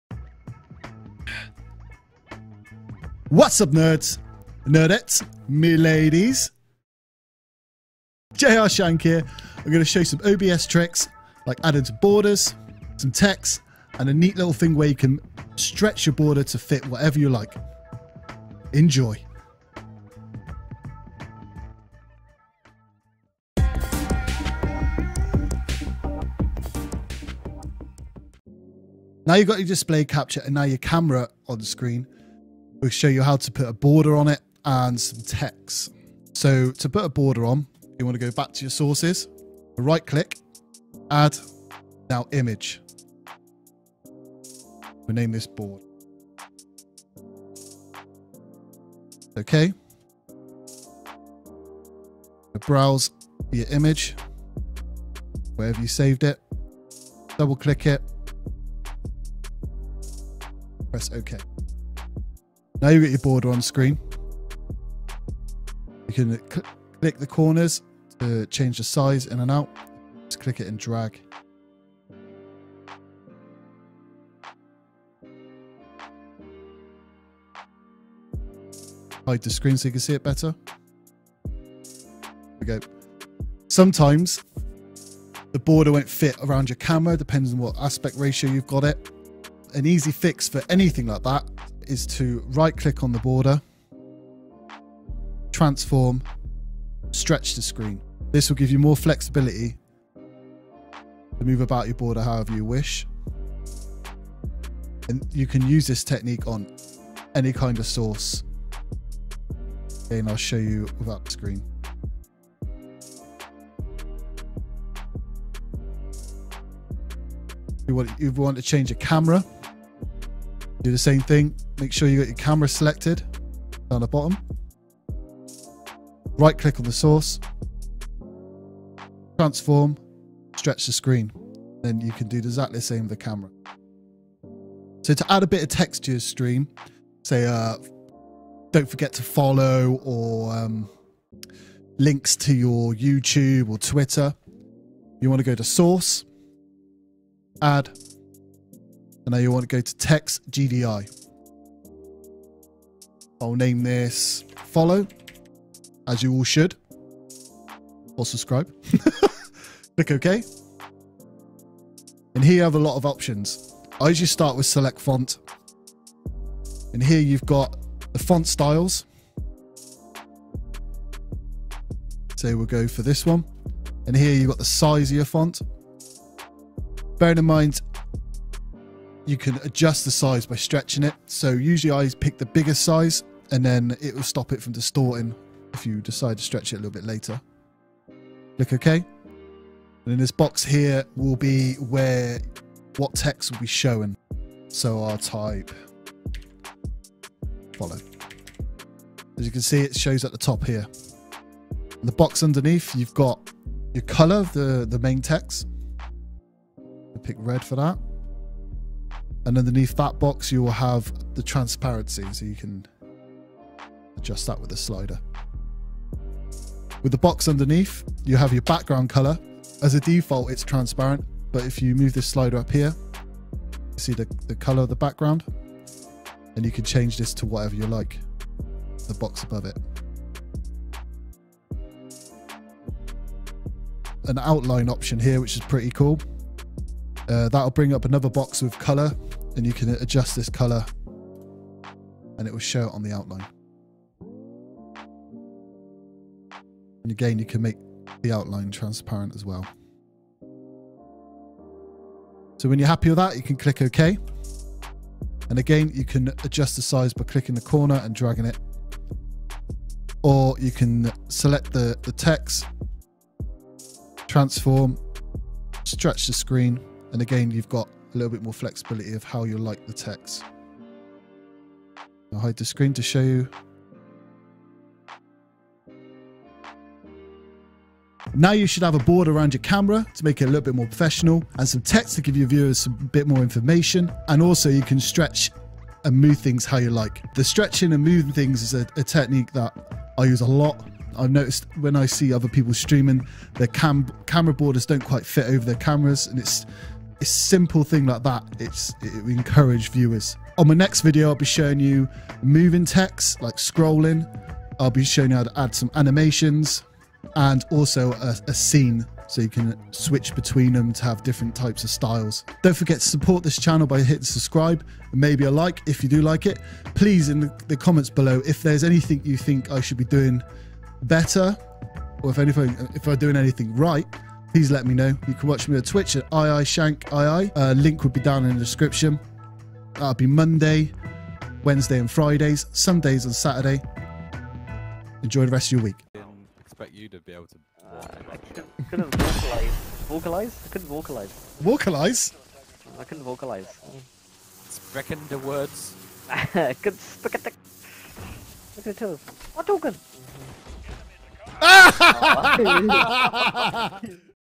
TWITCH OBS FOR DUMMIES Part 2 (Border tricks)
How to add borders and a tip on making them fit any camera size. Also how to add text to your stream